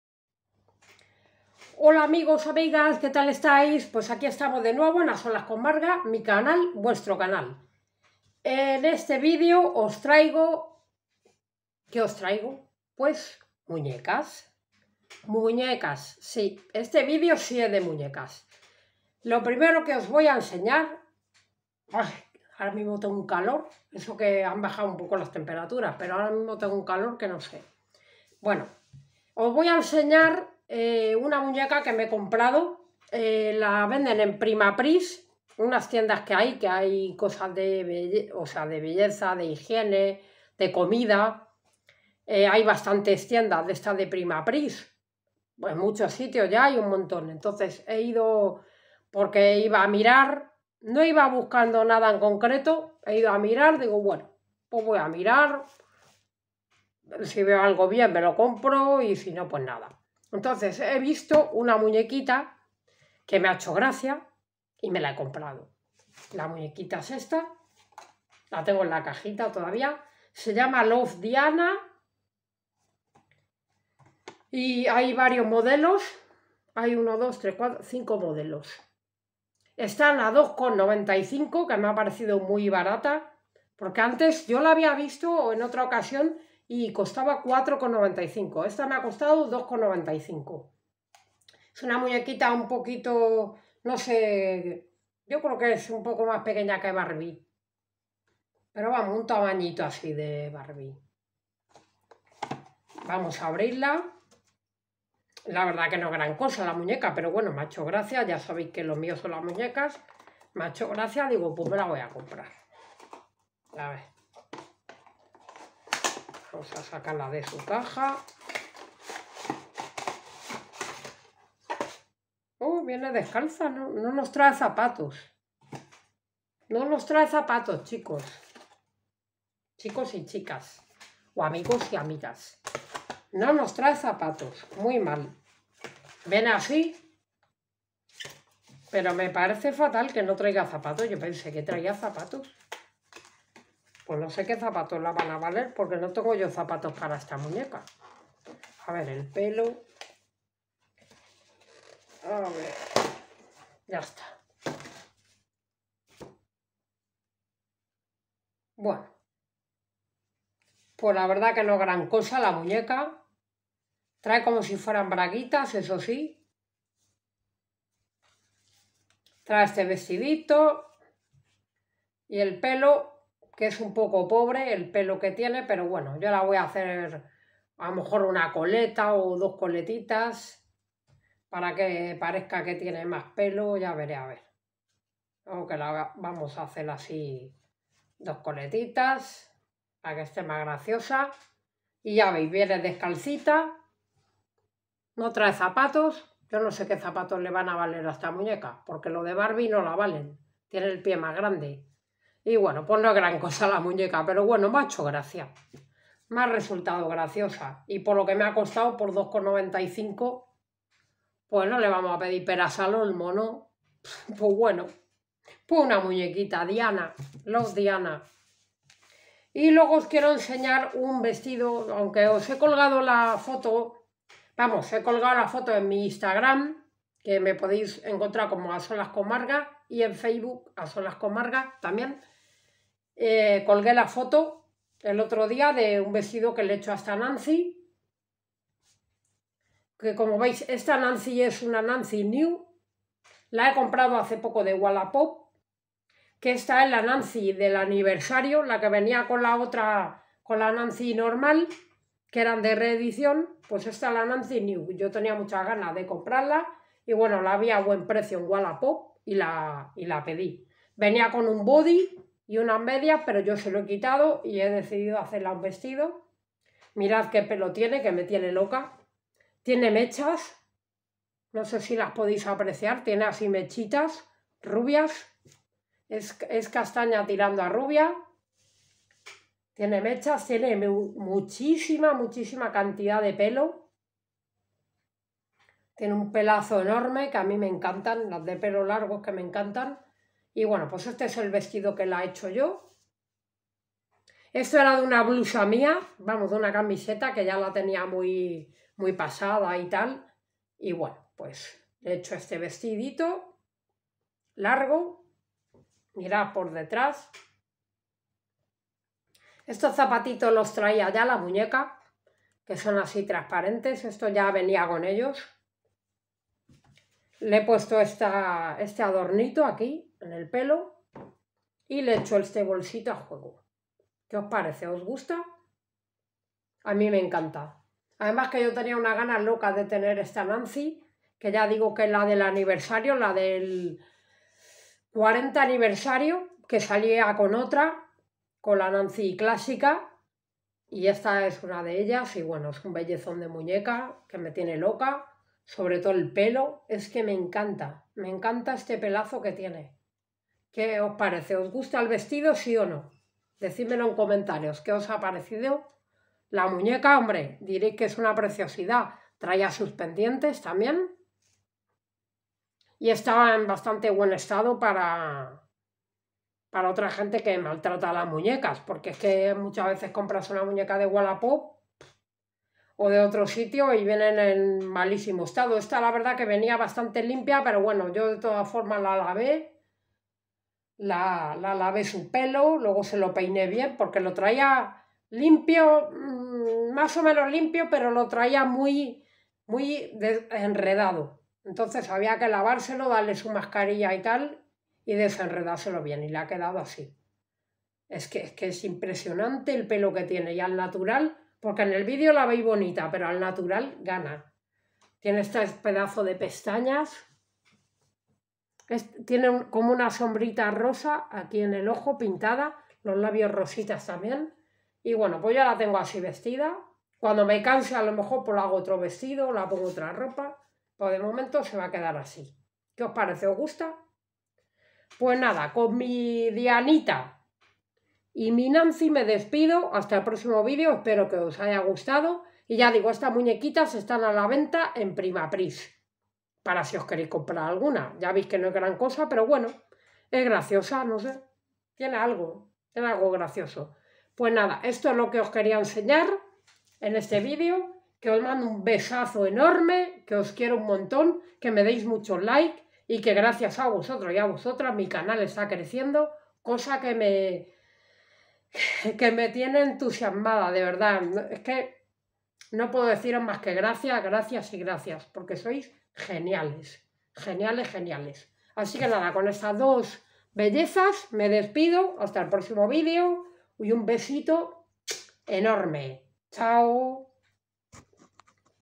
Muñeca Diana LOVE y Nancy new aniversario
os enseño una muñequita que me he comprado que se llama Diana LOVE y la Nancy new aniversario con un vestido hecho por mí, espero que os guste!!!